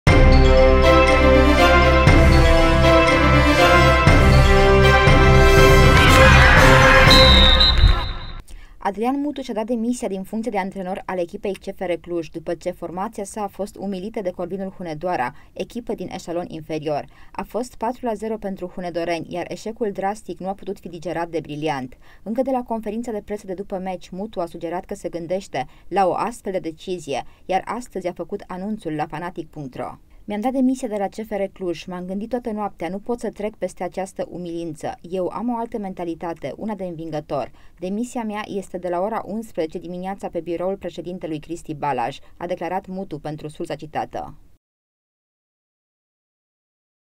Prea mare umilința! Adrian Mutu și-a dat demisia
Adrian Mutu și-a dat demisia din funcția de antrenor al echipei CFR Cluj, după ce formația sa a fost umilită de Corvinul Hunedoara, echipă din eșalon inferior. A fost 4-0 pentru hunedoreni, iar eșecul drastic nu a putut fi digerat de ,,briliant”. Încă de la conferința de presă de după meci Mutu a sugerat că se gândește la o astfel de decizie, iar astăzi a făcut anunțul la Fanatik.ro.